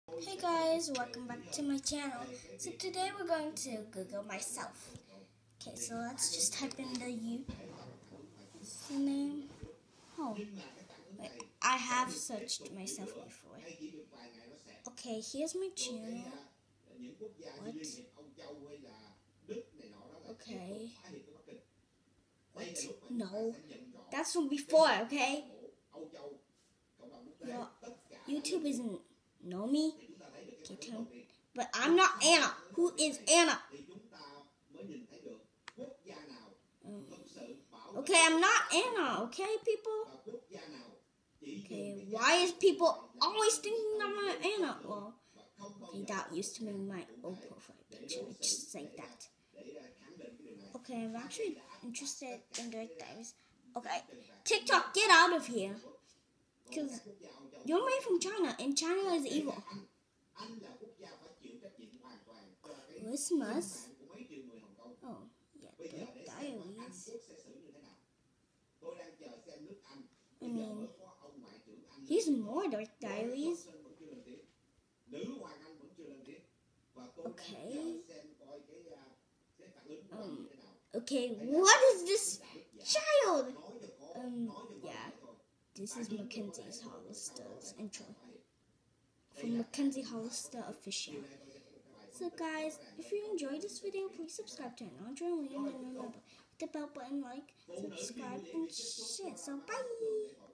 Googling myself (CRINGY ;-;)
Today I will be googling myself and also their is stuff that people think my name is Anna which makes me super mad! Also Happy 4th of July.

THIS WAS CRINGY